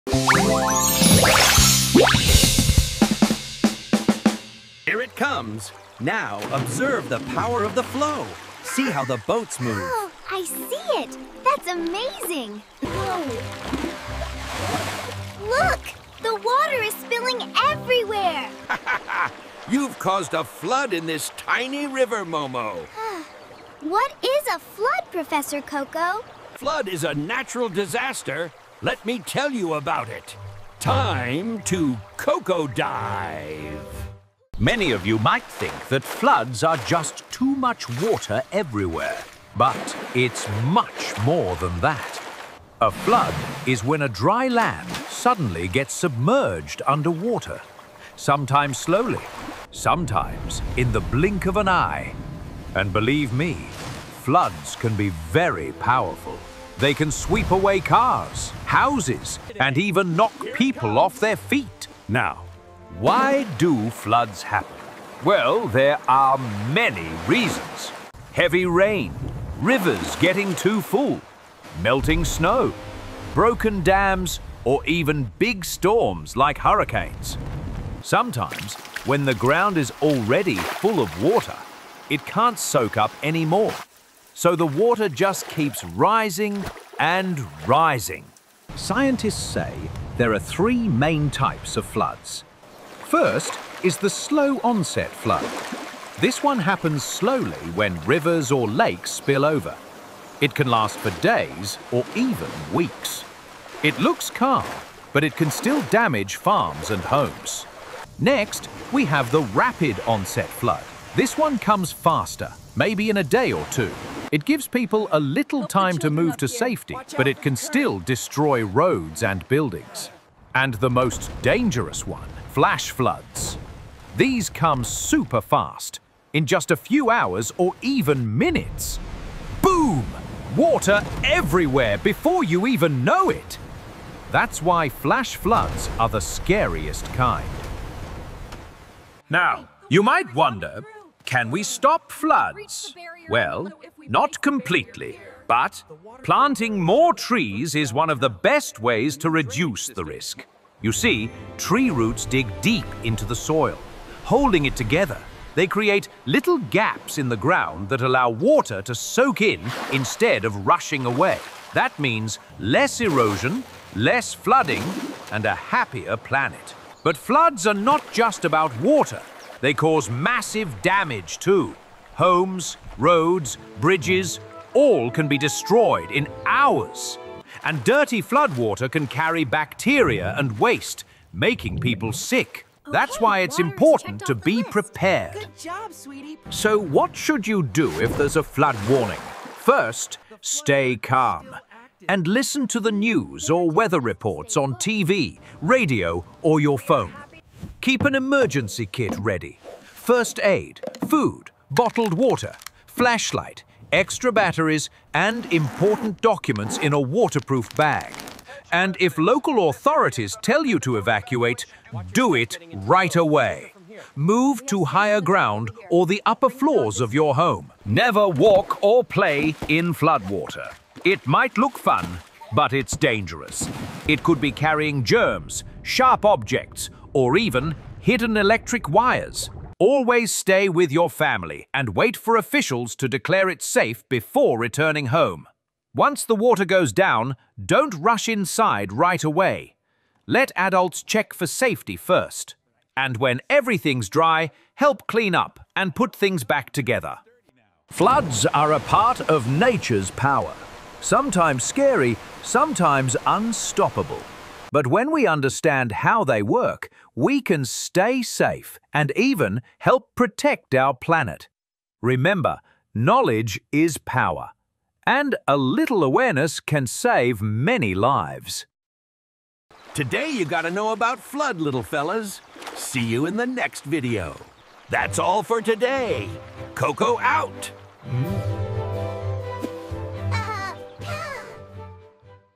What Are Floods? 🌊 | Learn How Floods Happen with Prof. Coco Show | Fun Science for Kids
Have you ever wondered what floods are and how they happen? 🤔 Join Professor Coco and Momo in this fun and exciting episode as they explain everything about floods for kids — what causes them, how they originate, and why they can sometimes be so destructive! 🌧️💧 Through colorful animations and simple explanations, this video helps kids understand the science behind floods in a fun and engaging way.

From learning how floods form during heavy rainfall 🌦️ to exploring their effects on nature and human life 🌍, this video is packed with knowledge and fun facts! It also includes important safety tips to stay safe during floods and some amazing facts that will surprise you! 😲💡

Perfect for students, young learners, and curious minds, this educational video turns a complex natural disaster into a simple, enjoyable learning experience. 👩‍🏫📚 So sit back, relax, and let Professor Coco and Momo take you on a journey to understand the fascinating world of floods! 🌊✨